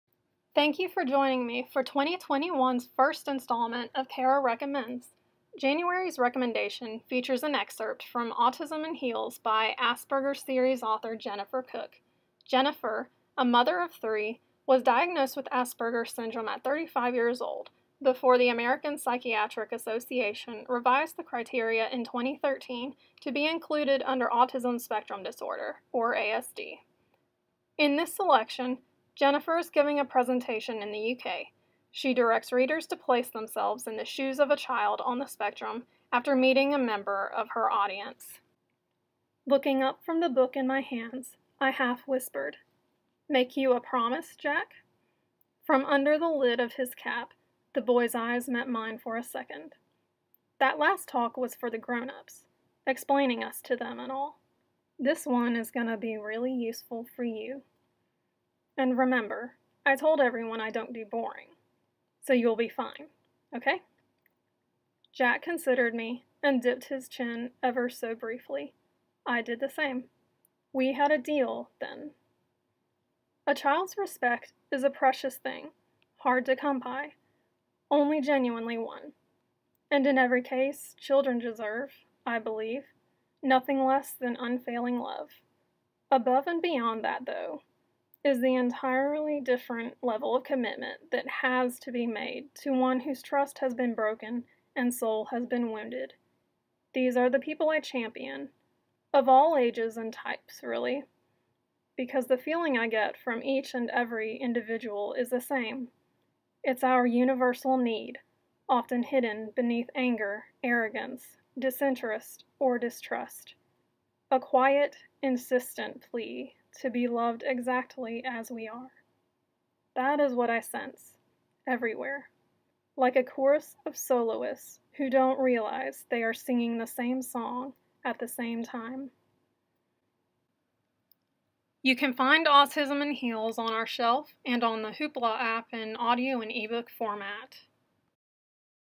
Kara Recommends...Autism in Heels
See Kara’s book trailer for Autism in Heels by best-selling Asperkids series author Jennifer O’Toole. Diagnosed at age thirty-five with Asperger’s, Jennifer shares her experiences and revelations from childhood to adulthood to motherhood.